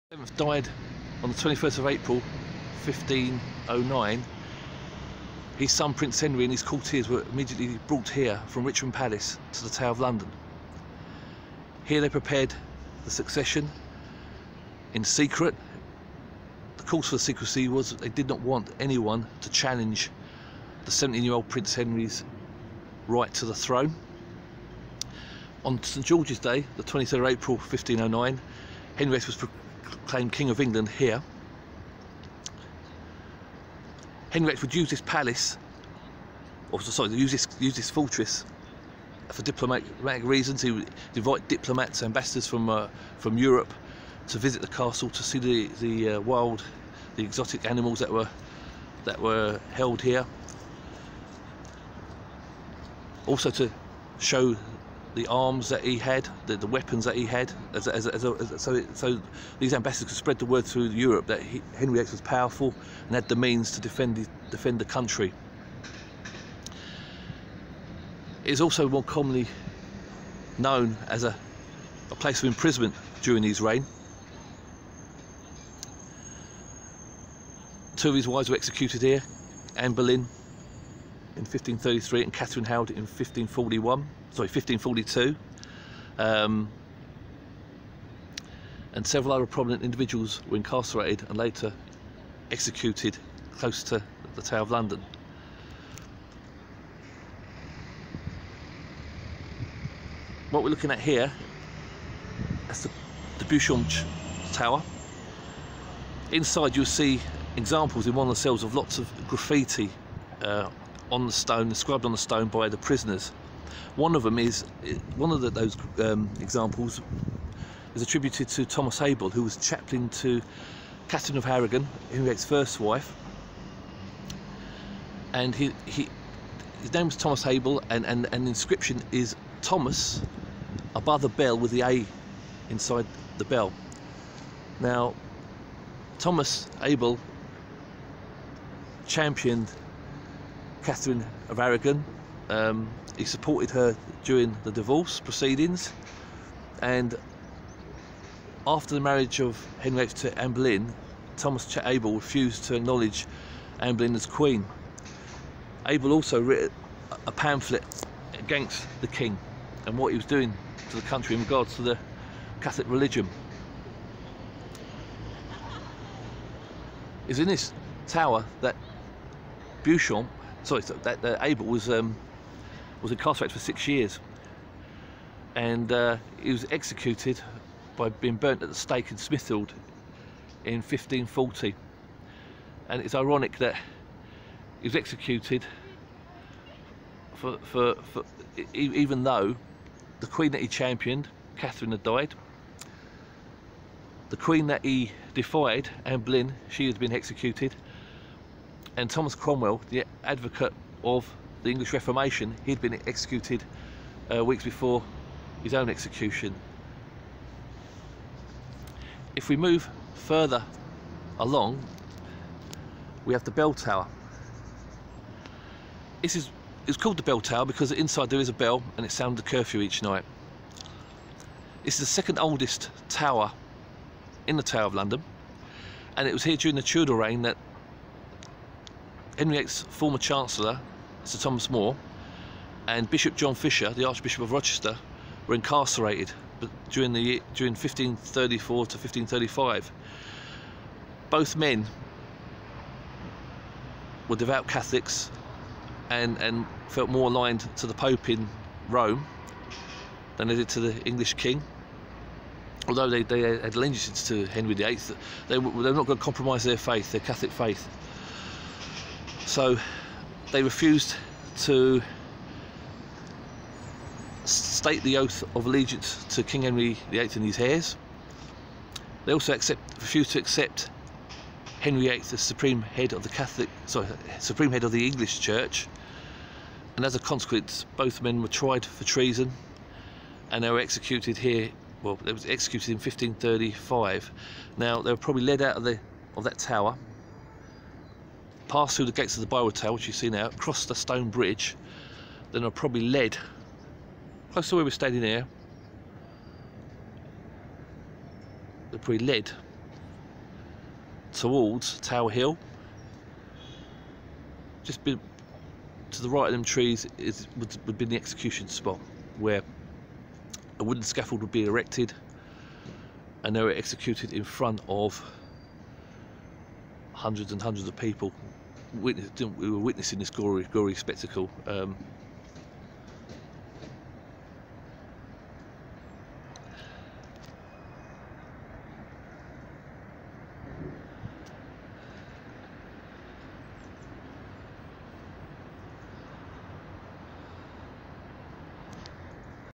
The Tower of London and Henry VIII
As featured in ‘Henry VIII in 100 Objects’ by Paul Kendall. Paperback available from Pen and Sword Books at the following link:

The Tower of London was associated with sadness for Henry because his mother, Elizabeth of York died during childbirth within its Royal Chambers in 1503. He stayed here prior to his coronation in 1509 and welcomed Anne Boleyn when she arrived by barge before her coronation in 1533. The Tower of London was used as a prison to hold political prisoners such as Sir Thomas More, Bishop John Fisher and Thomas Cromwell as well as his wives Anne Boleyn and Katherine Howard who were both executed within its walls. The Tower of London is therefore a visual symbol of Henry VIII’s reign of terror.